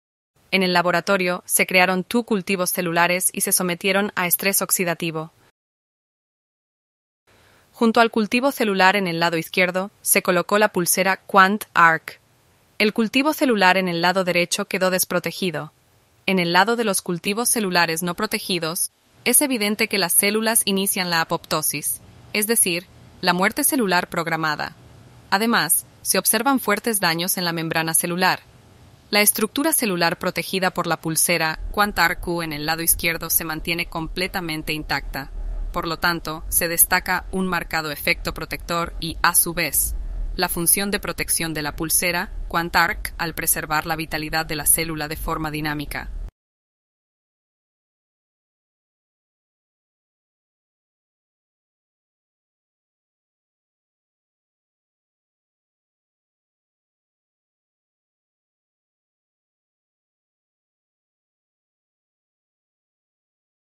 QUANT ARQ ESPAÑOL
Estrés oxidativo exógeno con y sin protección mediante la pulsera QUANT ARQ

Se registró la reacción de fibroblastos cultivados del tejido conectivo (línea celular L929) frente al estrés oxidativo exógeno mediante la adición de 2 mM de peróxido de hidrógeno al medio de cultivo, con y sin la protección de la pulsera QUANT ARQ, utilizando el microscopio invertido en contraste de fase.

Se tomó una microfotografía cada 15 segundos y luego se combinó cada sexta microfotografía para formar un videoclip. Durante la reproducción a 25 cuadros por segundo, esto significa una aceleración de 2.250 veces, es decir, 10 segundos en el video equivalen a casi 24 horas en la realidad. Los videoclips tienen una duración de 36 segundos, por lo que la reproducción en un videoclip representa los eventos celulares de casi 24 horas en tiempo real.

Es claramente visible que en el cultivo celular sin la protección de la pulsera QUANT ARQ, las células se contraen lentamente debido al estrés oxidativo, adoptando una forma redondeada y quedando conectadas al sustrato (= placa de cultivo) solo con unas pocas extensiones citoplasmáticas. Con el tiempo, se observa un aumento de células que entran en apoptosis (= muerte celular programada) y luego yacen en el lecho celular como partículas irregulares. En muchas células, los daños en las membranas conducen a una densificación y granulación intensas del citoplasma.

En el cultivo protegido por la pulsera QUANT ARQ, los cambios celulares son escasos. Se observa de manera más pronunciada la aparición moderada de apoptosis. El conjunto de la capa celular permanece intacto, incluso si los espacios entre las células se vuelven un poco más grandes.

Por lo tanto, se visualiza de forma dinámica el efecto protector pronunciado de la pulsera QUANT ARQ en la vitalidad de los fibroblastos del tejido conectivo.